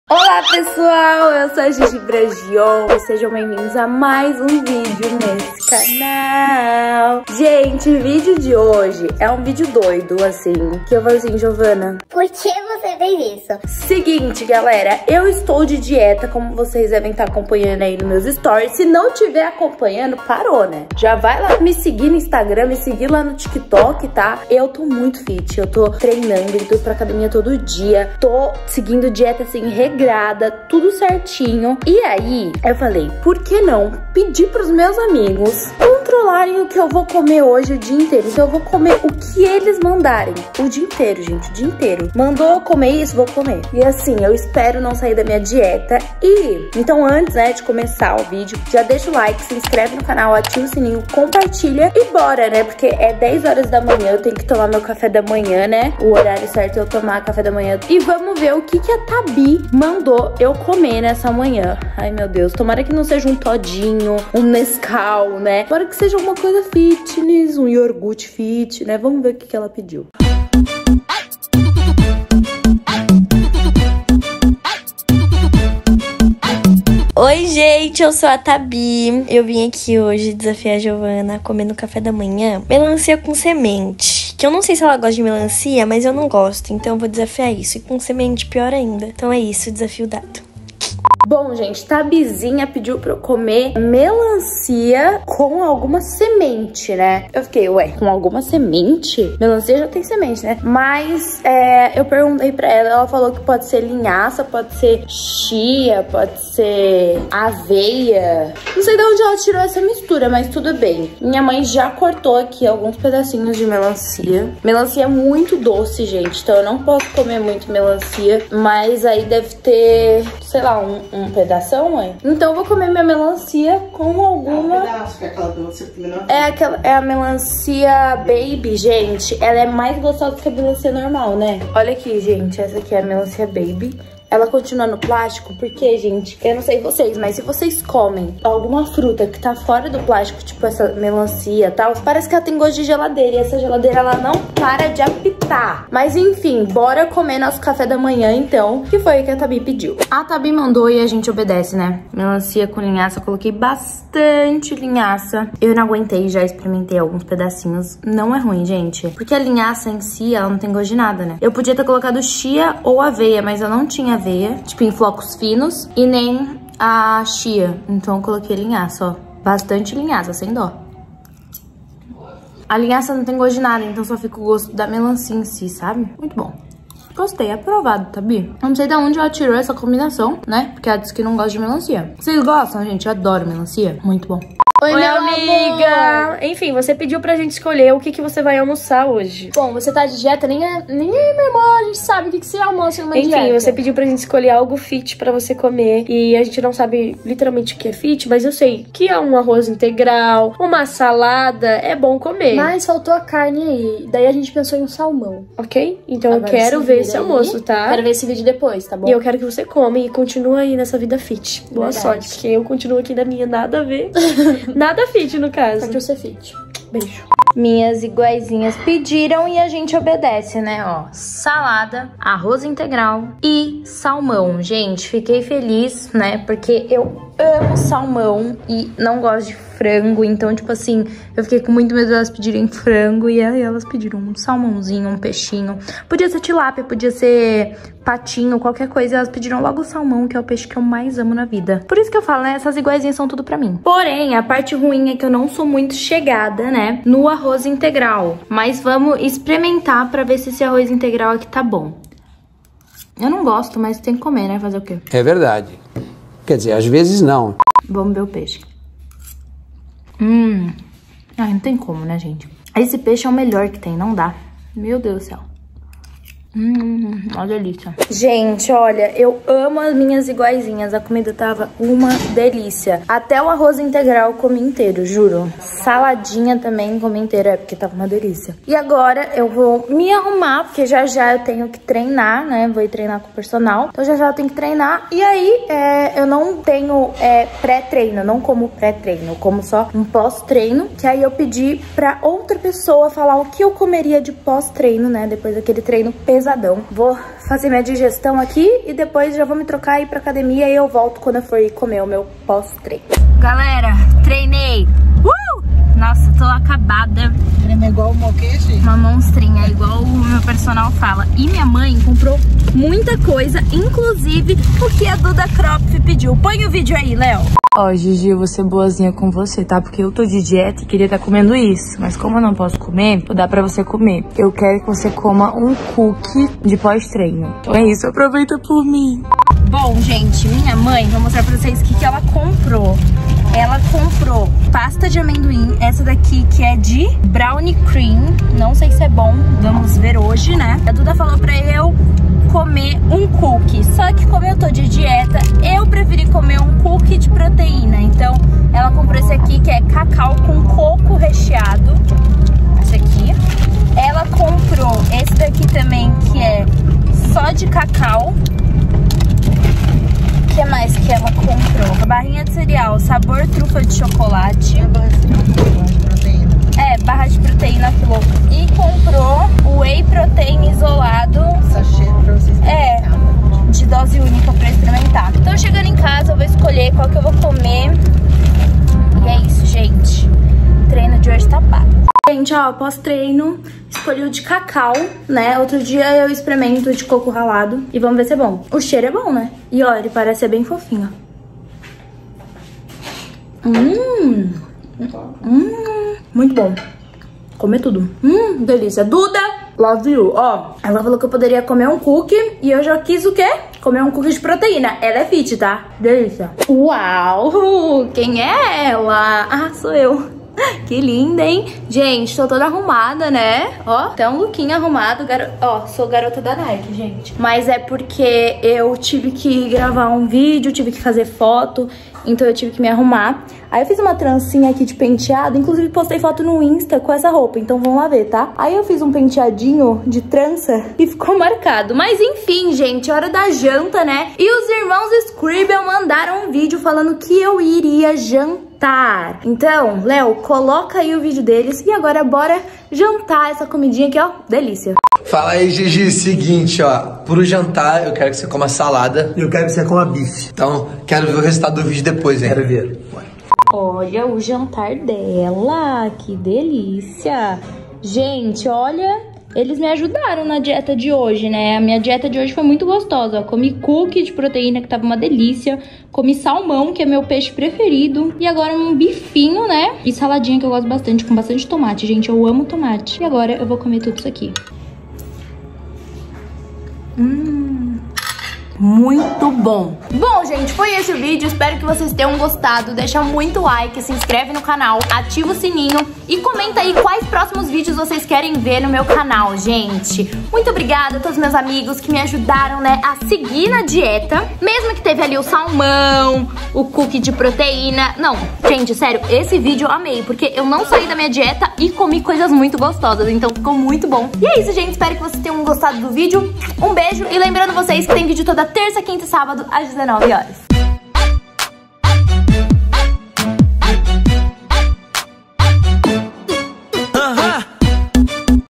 24 HORAS COMENDO TUDO QUE MEUS AMIGOS YOUTUBERS MANDARAM!! ft. Eu Você e Nos, Taby, Scribel e Kropf
Oii gente!!! No vídeo de hoje, olha só eu que esses meus amigos aprontaram comigo, será que deu bom gente? Gostou do vídeo?